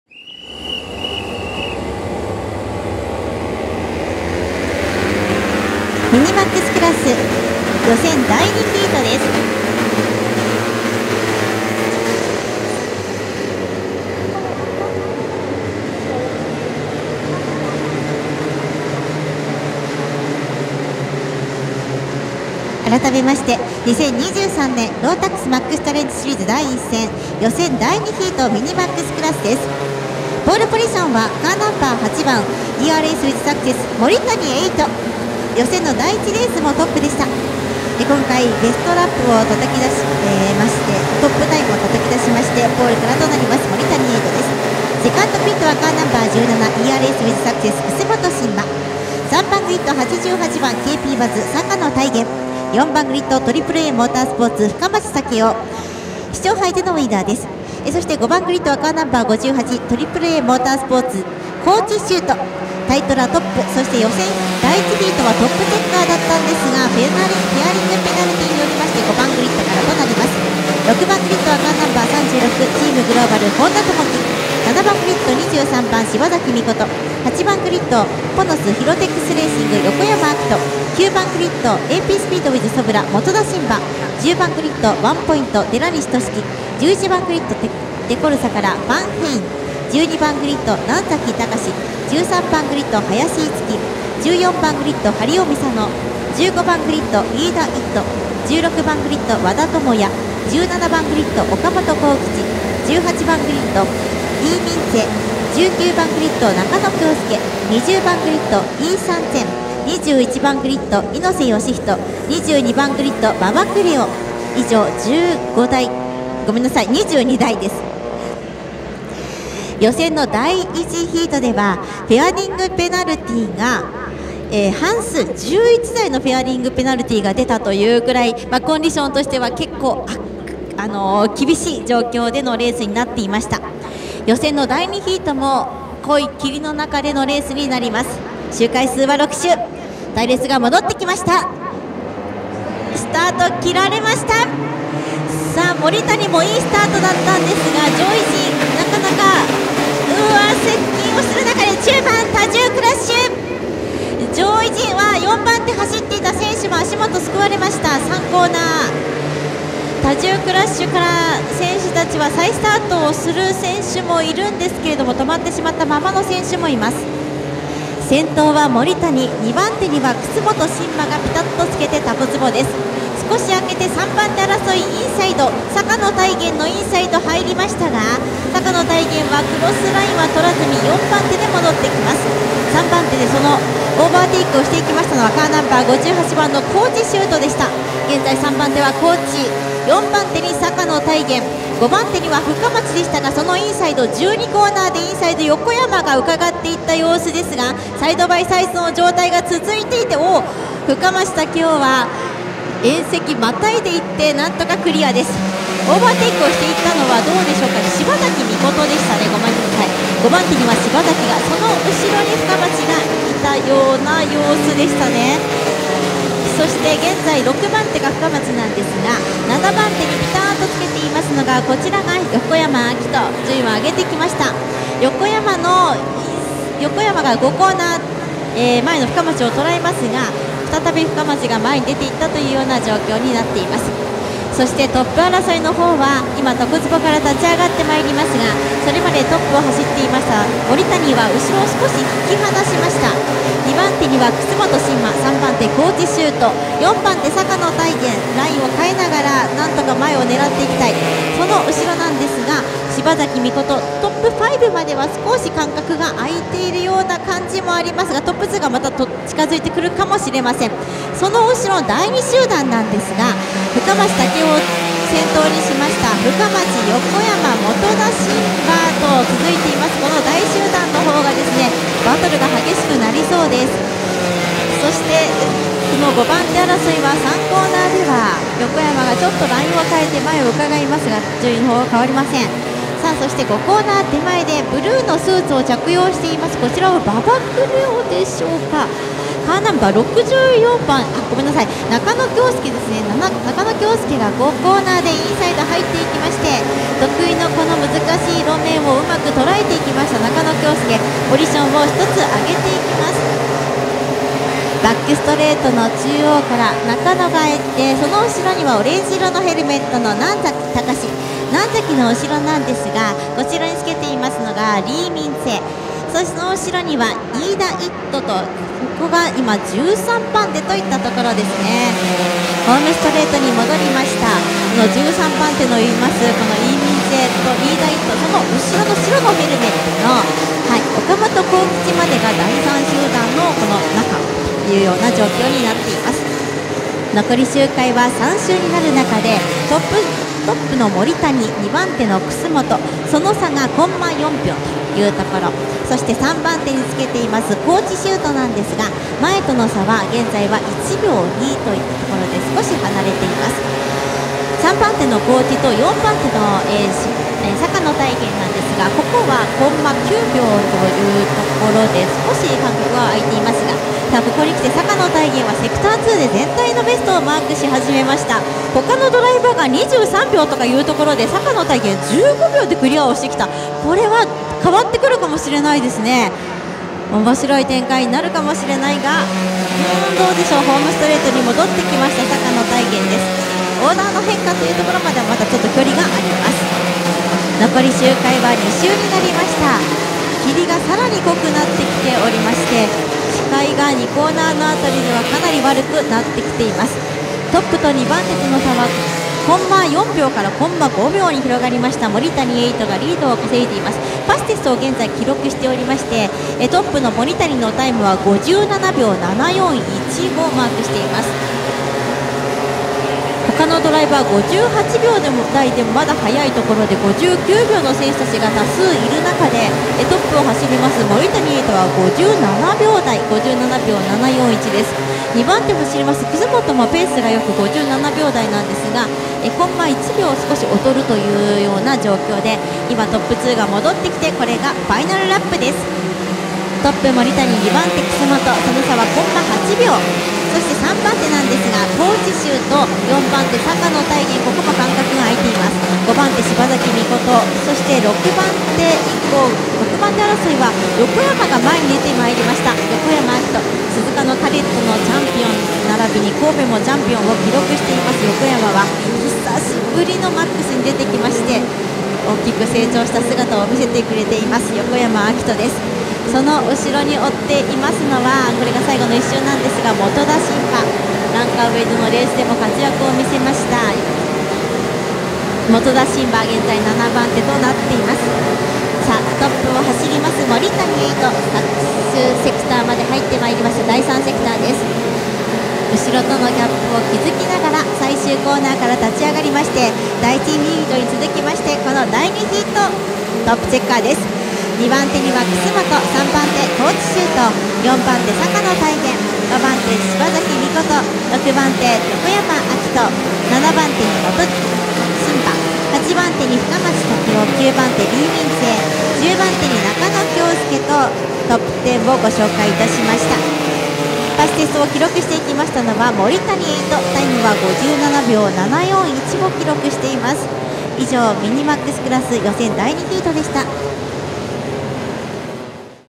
ROTAX MAX 瑞浪シリーズ2023 第1戦 Mini MAX 予選ヒート2
開催日：2023年3月25日(土)
開催場所：フェスティカサーキット瑞浪（岐阜県）
大会名：ROTAX MAX CHALLENGE 瑞浪シリーズ2023 第1戦

大会概要、正式結果などの情報はこちら